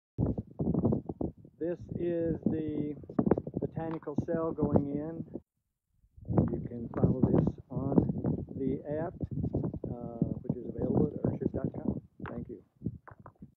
Day 12 Earthship Encounter
This is DAY 12: This video is showing the botanical cell dug out, lined with six mill plastic (two layers), then EPDM, and then we will be putting in the standpipes, the pumps and the gravel and rock on next workday! We will soon have a diagram posted on and labeling all of those things: You can have the drawings and plans of this building on our app: